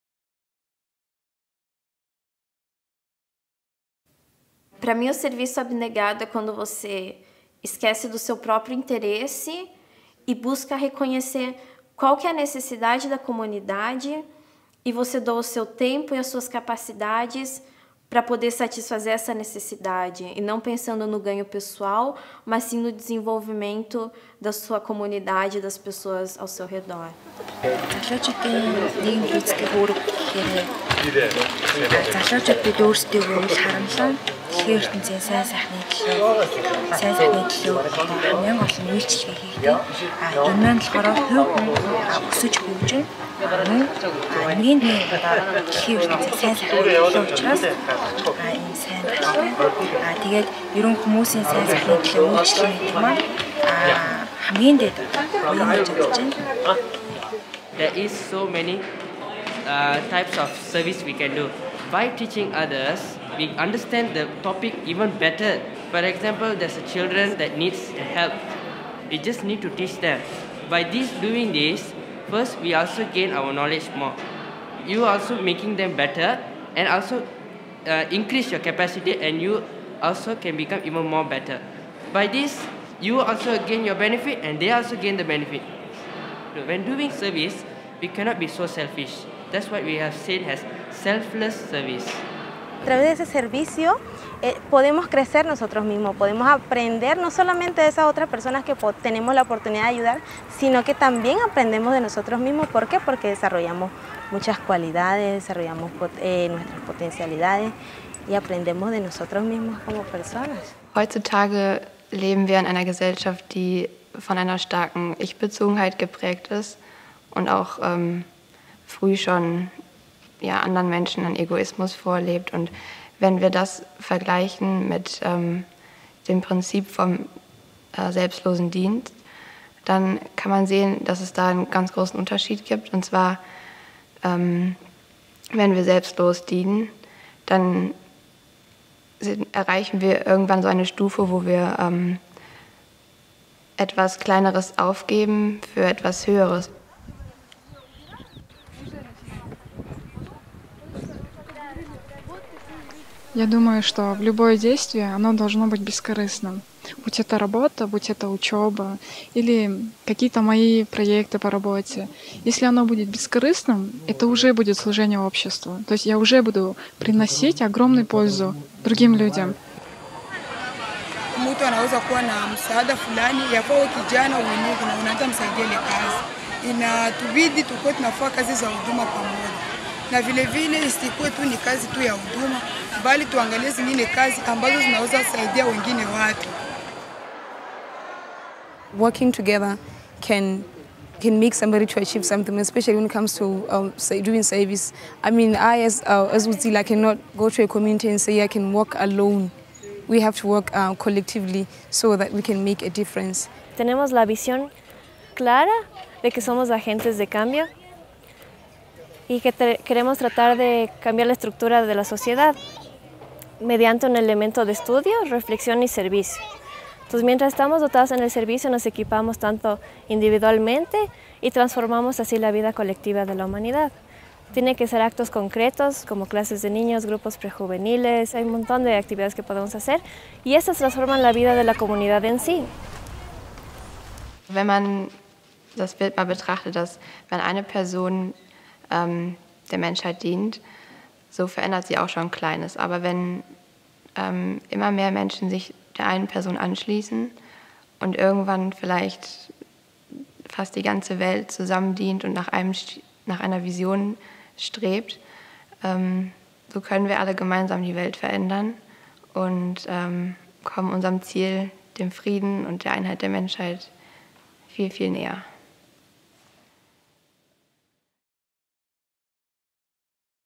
Servire L'Umanità Il Servizio Altruistico (4-10)
Riflessioni sui temi dei Convegni Bahá'í per i Giovani, video 4 di 10 sul tema del servizio altruistico.
Preparato dal Centro Mondiale Bahá'í.